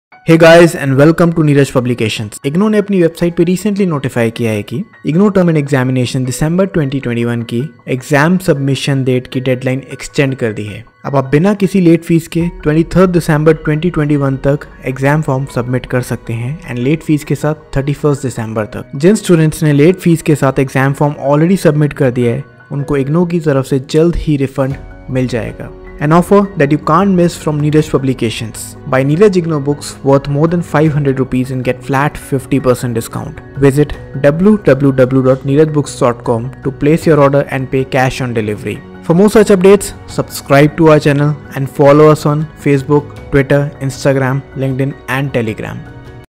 IGNOU Exam Form Submission Date Extended (TEE December 2021) | NEERAJ Books
IGNOU has extended the IGNOU TEE December 2021 exam form submission date. The exam form submission date has been extended till December 23, 2021 up to 12 am and with late fees till December 31, 2021. Candidates can check the official notice on the official site of IGNOU

Neeraj Publications is providing an incredible offer on all its Books. Buy Books for more than Rs.